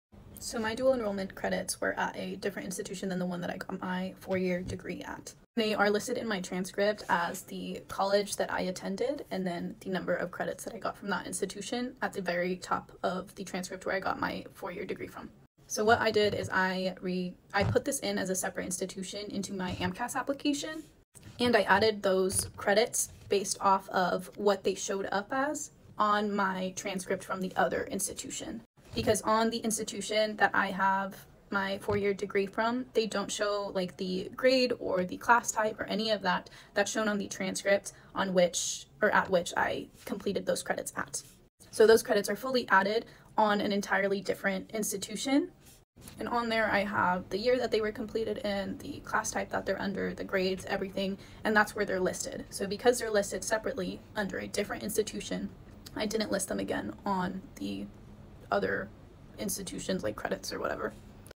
Adding dual enrollment courses in AMCAS!
Hey! I didn’t take my dual enrollment courses at a junior college, but from what I understand, if you earned the credits at a community college, you should list that institution as a “junior college” in AMCAS. Even if the credits transferred to your university, AMCAS wants the original institution listed separately. Hope that helps!